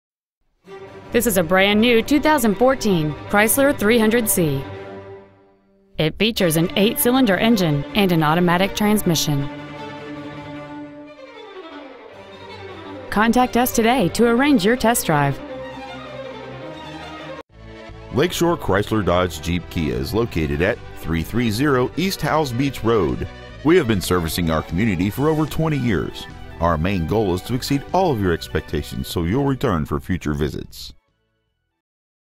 2014 Chrysler 300C Slidell LA 70461
Year : 2014
 Make : Chrysler
 Model : 300C
 Engine : V8
 Trans . : Automatic
 Exterior : White
 Miles : 17
 Interior : Beige
 Stock : 75504

 330 E. Howze Beach Road

 2014 Chrysler 300C Slidell LA